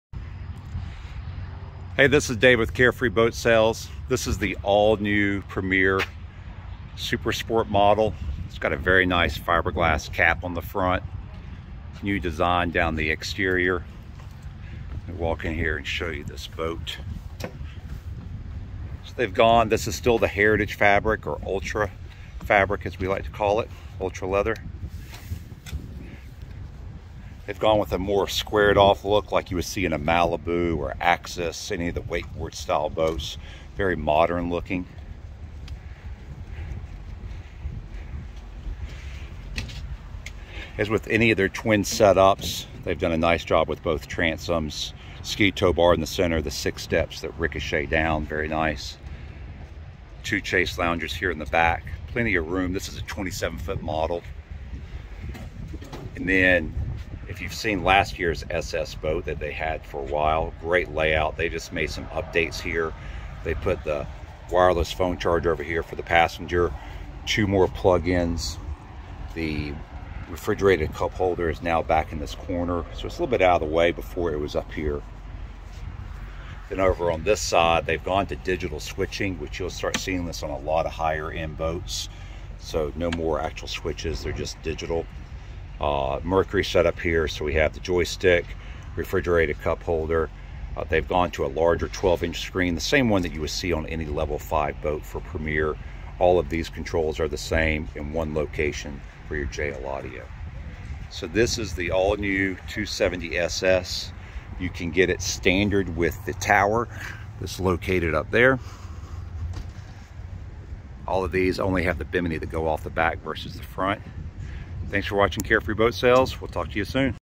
Brand new Premier Pontoons 2023 270 Supersport with 900HP
All new Premier Pontoons Super Sport Model.   it's awesome!!  Only the Best by Premier Pontoons